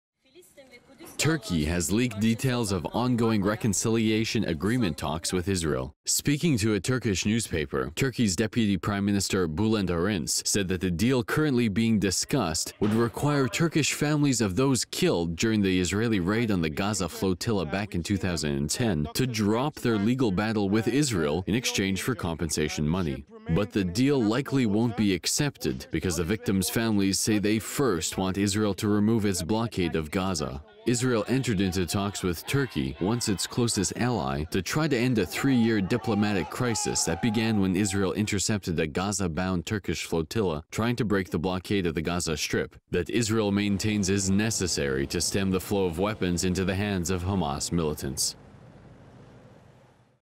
Flotilla raid talks: Turkish families must drop legal claims against Israel before receiving cash
Turkey has leaked details of ongoing reconciliation agreement talks with Israel.  Speaking to a Turkish newspaper, Turkey's Deputy Prime Minister Bulent Arincsaid that the deal currently being discussed would require Turkish families of those killed during the Israeli raid on the Gaza flotilla back in 2010 to drop their legal battle with Israel in exchange for compensation money.